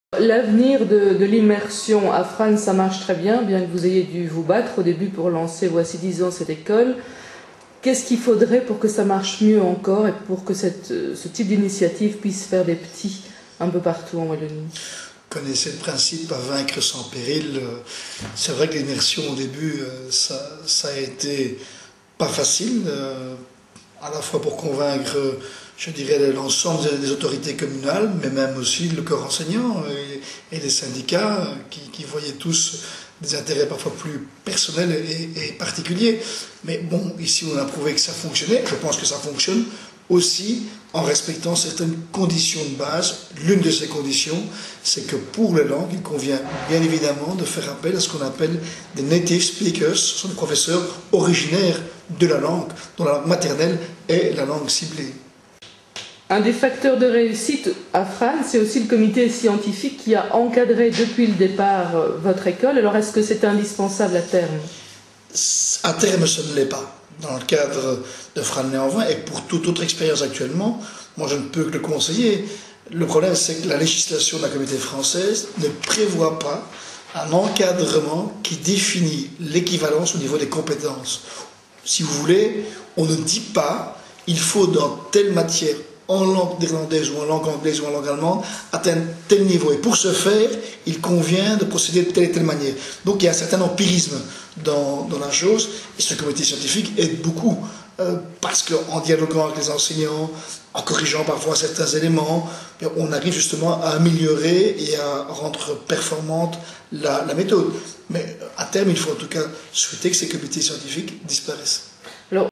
Les deux piliers de la reussite de l'immersif à Frasnes
encadrement universitaire et professeurs "native speaker"